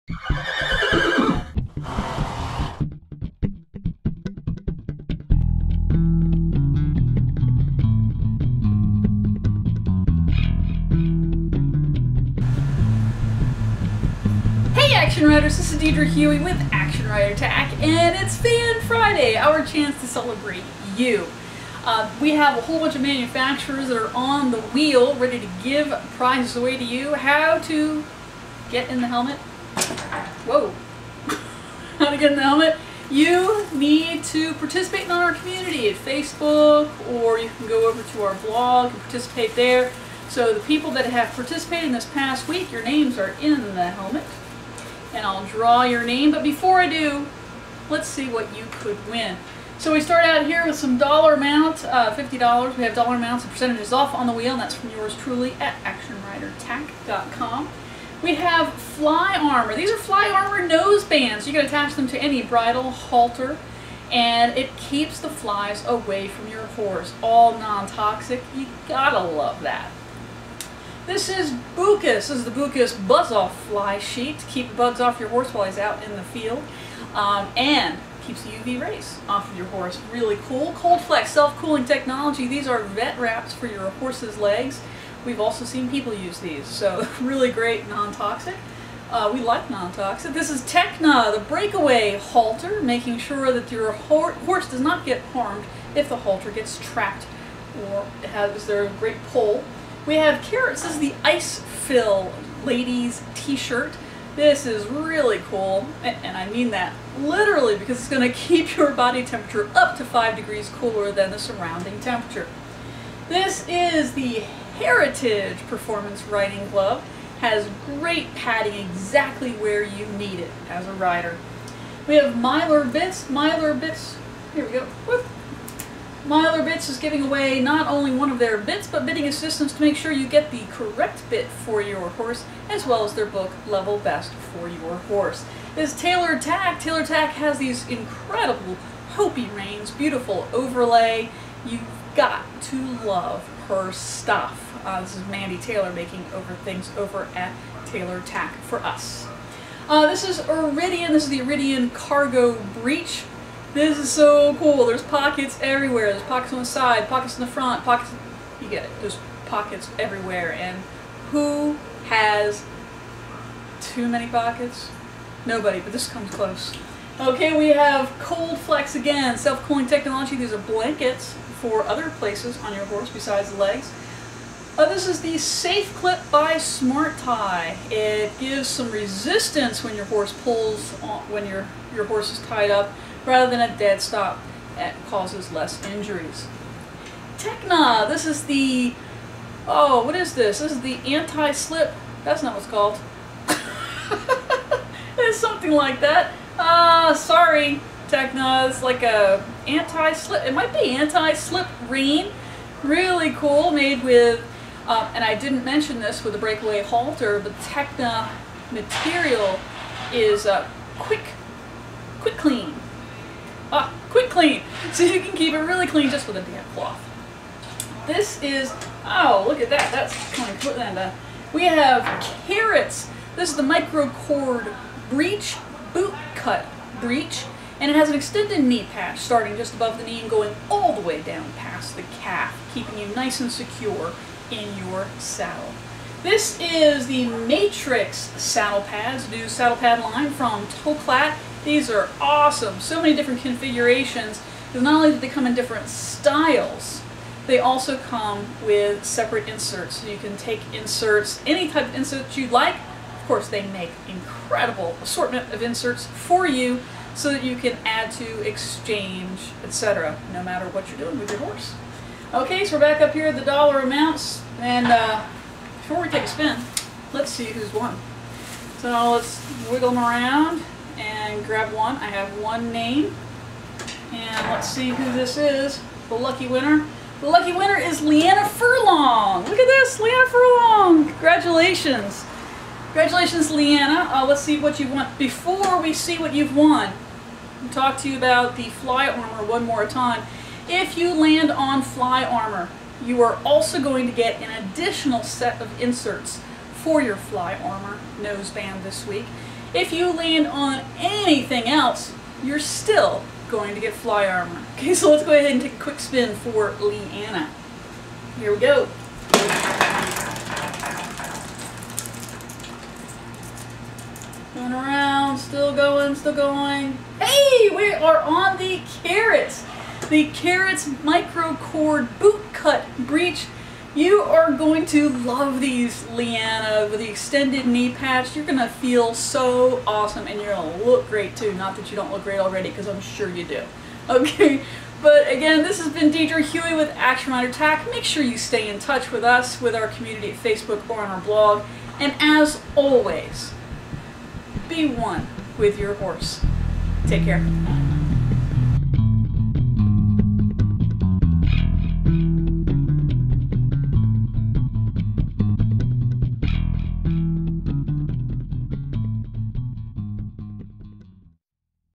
Customer Wins Kerrits Microcord Bootcut Breeches and Fly Armor Noseband
- This week on Fan Friday, Leiana Furlong's name was selected and with a big spin of the wheel, Leiana is the recipient of Kerrits Microcord Bootcut Breeches with Extended Kneepatch as well as Fly Armor Nosebands for her horse! Want to be a winner? Become a part of the community on Facebook or on their blog
Be One With Your Horse!(TM)